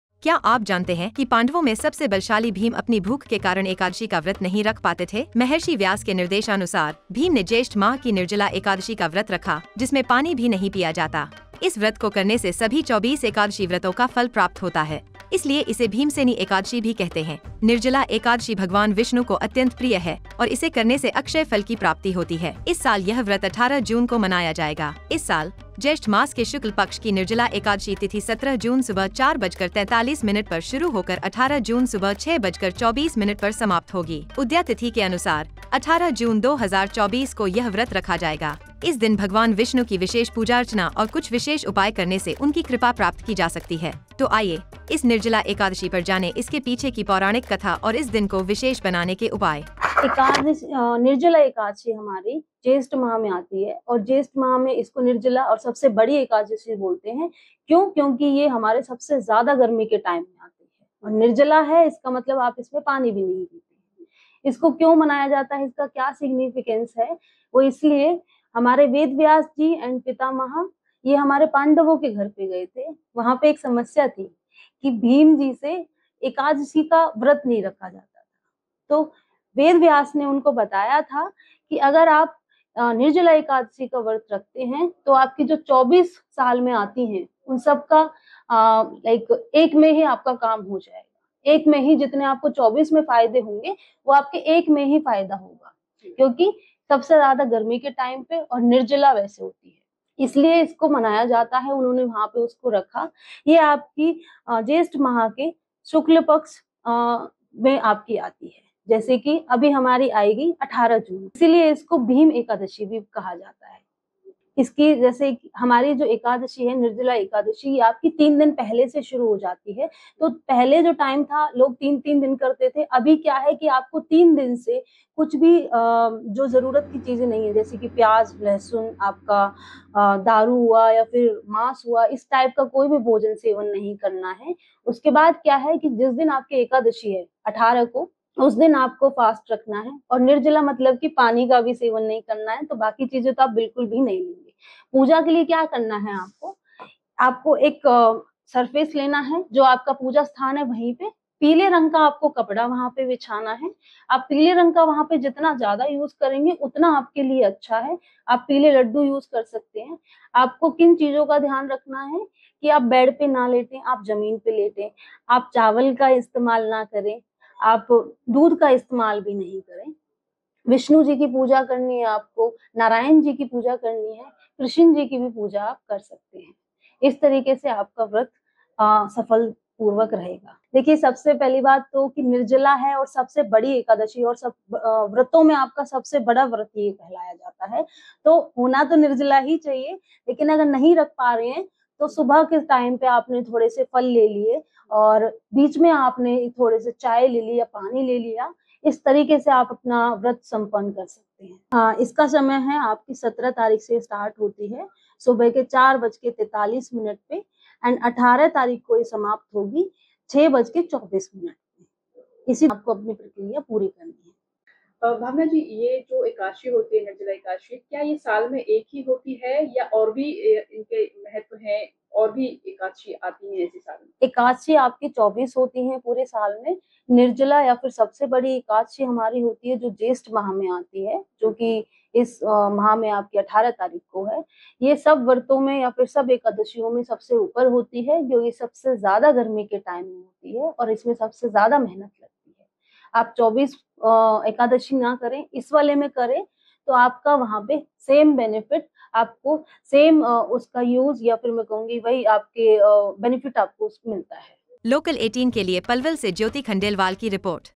Nirjala Ekadashi 2024: गंगा दशहरा पर नीलकंठ के दर्शन का विशेष महत्व!
क्या आप जानते हैं कि पांडवों में सबसे बलशाली भीम अपनी भूख के कारण एकादशी का व्रत नहीं रख पाते थे? महर्षि व्यास के निर्देशानुसार, भीम ने ज्येष्ठ माह की निर्जला एकादशी का व्रत रखा, जिसमें पानी भी नहीं पिया जाता। इस व्रत को करने से सभी 24 एकादशी व्रतों का फल प्राप्त होता है, इसलिए इसे भीमसेनी एकादशी भी कहते हैं।

Do you know that Bhima, the strongest among the Pandavas, could not observe the fast of Ekadashi due to his hunger? As per the instructions of Maharishi Vyas, Bhima observed a fast on Nirjala Ekadashi of Jyeshtha month, in which even water is not drunk. By observing this fast one gets the results of all the 24 Ekadashi fasts, hence it is also called Bhimseni Ekadashi.